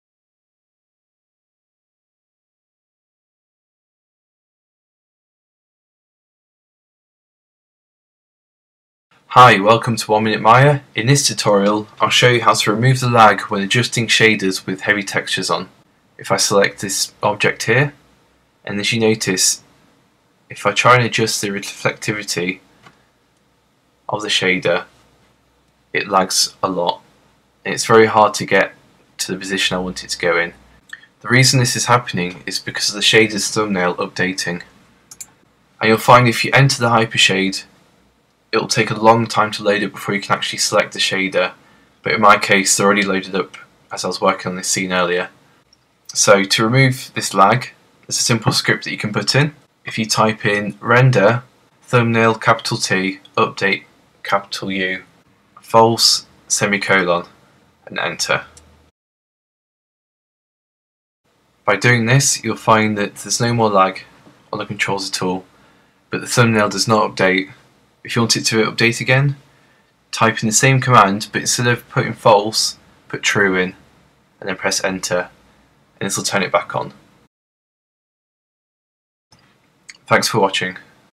How to reduce lag within the hyper shade or on shaders in Maya 2013
In this lesson I am going to show you how to reduce lag within the hyper shade or on shaders within Maya.
Towards the end of a project you may find that your scene will lag a lot when adjusting shaders for render previews. This tutorial will show you the reason for the lag and how to stop it lagging.